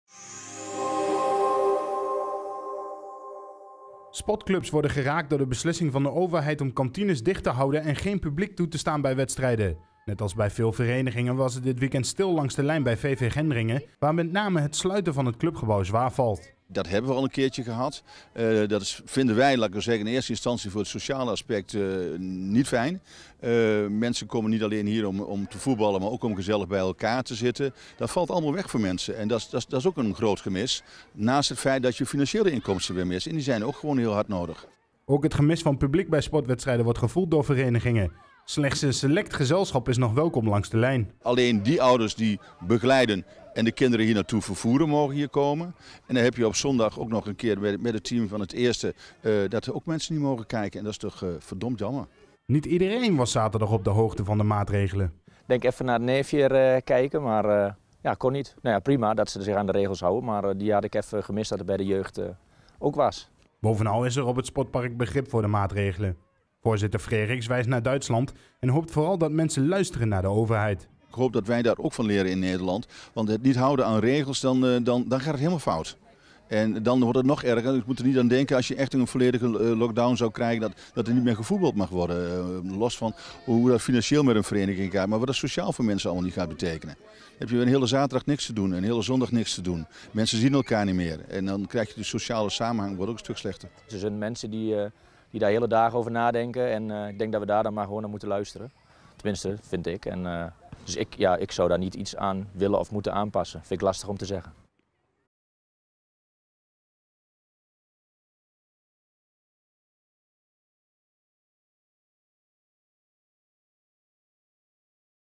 Stilte langs de lijn doet pijn bij sportverenigingen als VV Gendringen
Sportclubs worden geraakt door de beslissing van de overheid om kantines dicht te houden en geen publiek toe te staan bij wedstrijden. Net als bij veel verenigingen was het dit weekend stil langs de lijn bij VV Gendringen, waar met name het sluiten van het clubgebouw zwaar valt.